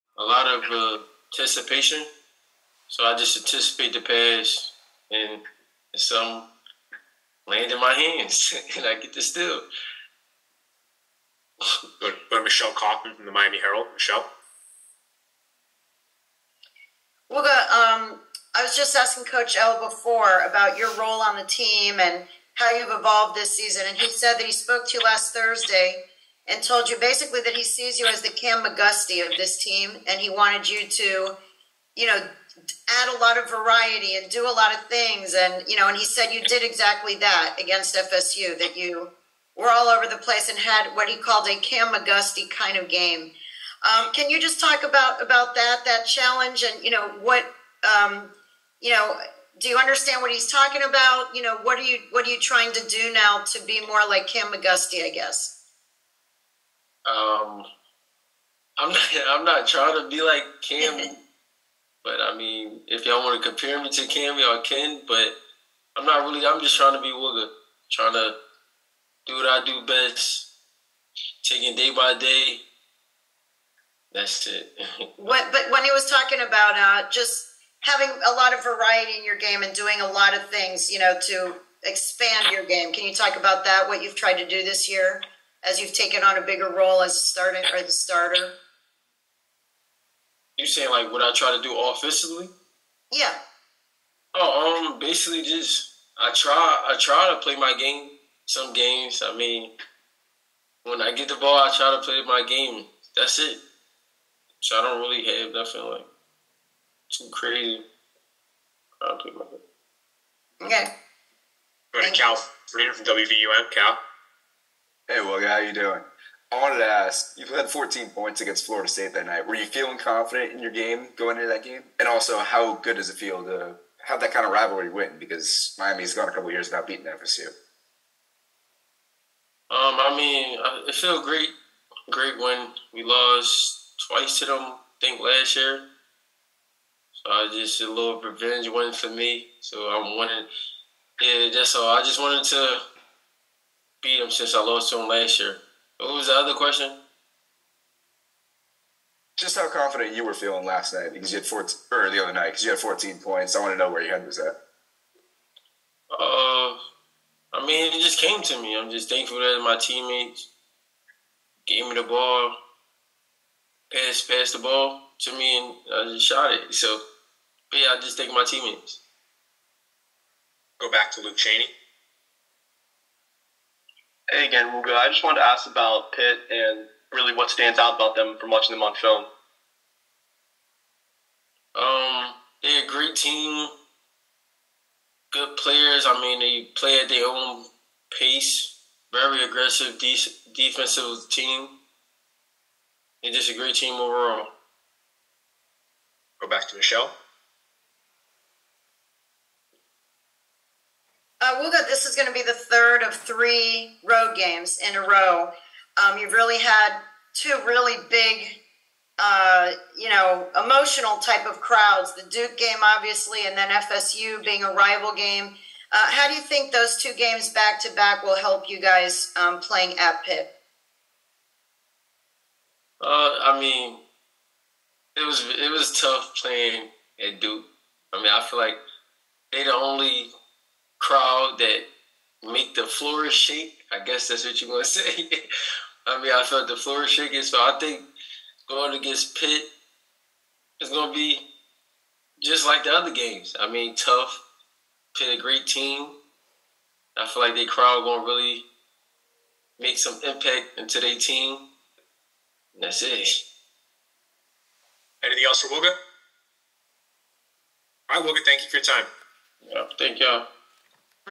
Wooga Poplar (Miami Guard) Talks to Media Ahead of Game At Pitt
for all things University of Miami Football and other sports.